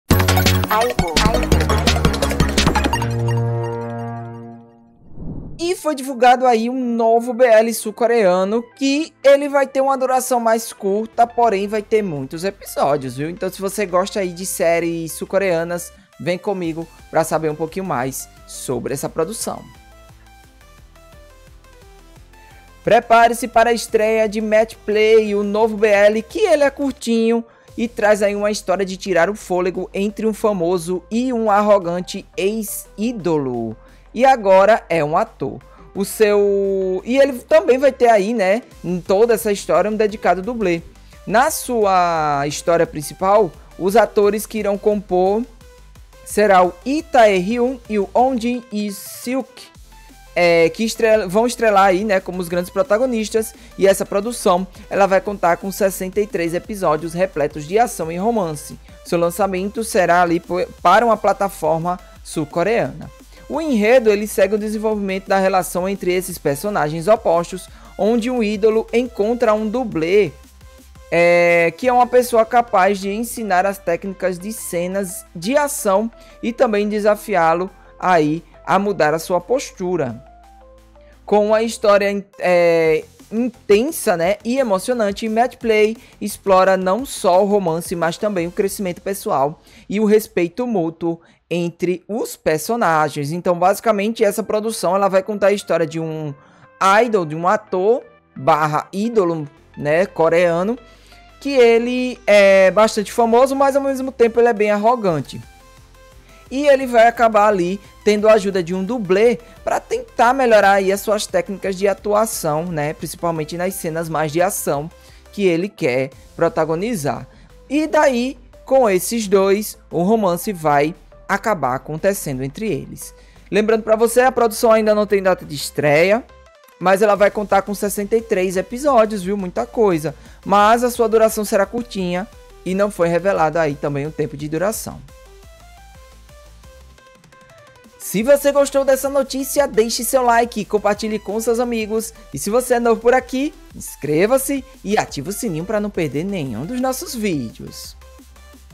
🚨Novo BL coreano 'Match Play' promete emocionar com romance entre ídolo e dublê
Prepare-se para a estreia de Match Play (매치플레이), o novo BL curto que traz uma história de tirar o fôlego entre um famoso e arrogante ex-ídolo, agora ator, e seu dedicado dublê. Lee Tae Hyung e Oh Jin Seok estrelam esta produção coreana, que conta com 63 episódios repletos de ação e romance, lançados na plataforma Vigloo. O enredo segue o desenvolvimento da relação entre esses personagens opostos, onde o ídolo encontra no dublê uma pessoa capaz de ensinar as técnicas das cenas de ação e desafiá-lo a mudar sua postura. Com uma história intensa e emocionante, Match Play explora não só o romance, mas também o crescimento pessoal e o respeito mútuo que surge entre eles.

🔴 Estamos ao vivo de segunda a sexta, às 19h, com tudo que acontece no universo BL!